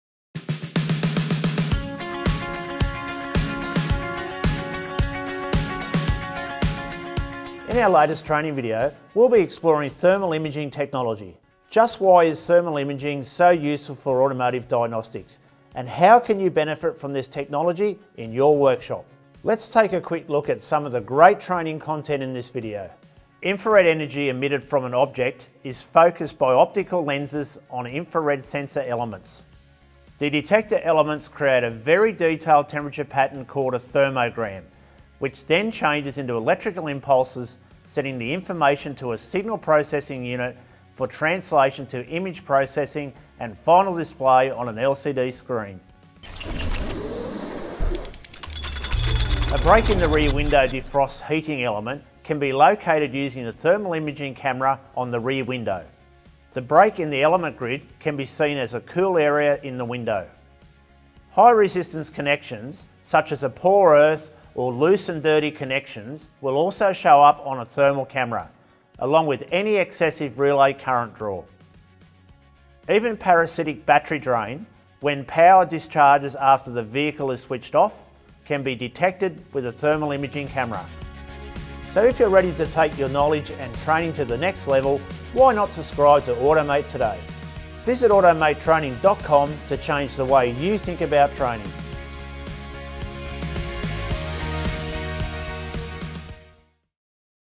Automotive Thermal Imaging & Diagnostics Training Module Trailer
As thermal imaging becomes increasingly common in the automotive service and repair space, technicians should prepare to take advantage of this technology and the opportunity it creates for faster and more accurate diagnostics.

Looking to keep your automotive knowledge up to date?